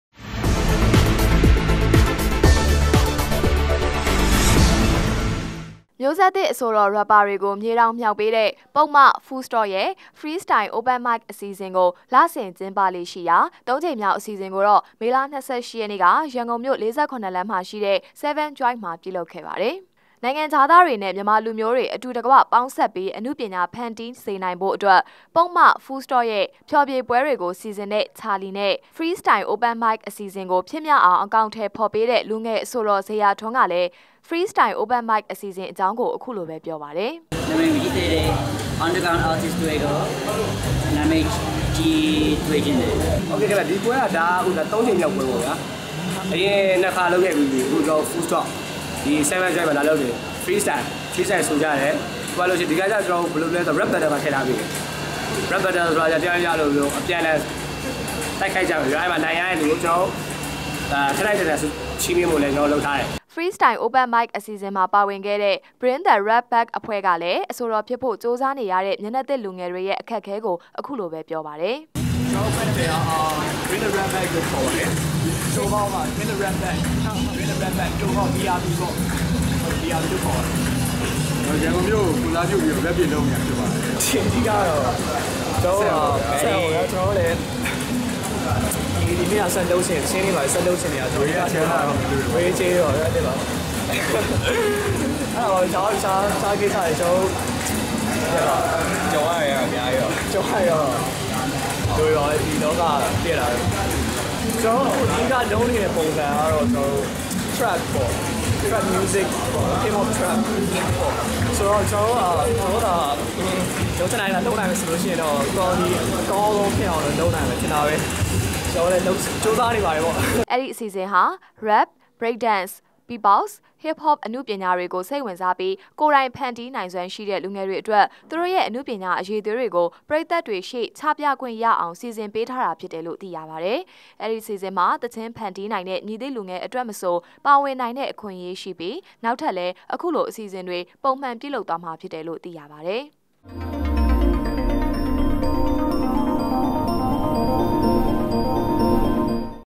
မ်ိဳးဆက္သစ္ Rapper အဆိုေတာ္မ်ားကိုေျမေတာင္ေျမွာက္ေပးသည့္...
မ်ိဳးဆက္သစ္အဆိုေတာ္၊ Rapper ေတြကိုေျမေတာင္ေျမွာက္ေပးတဲ့ ပုဒ္မ (Full Stop) ရဲ႕ Freestyle Open Micအစီအစဥ္ကိုလစဥ္က်င္းပေလ့ရွိရာသံုးႀကိမ္ေျမာက္အစီအစဥ္ကိုေမလ ၂၈ ရက္ေန႔ကရန္ကုန္ၿမိဳ႕ ၄၇ လမ္းမွာရွိတဲ့ 7 joint မွာျပဳလုပ္ခဲ့ပါတယ္။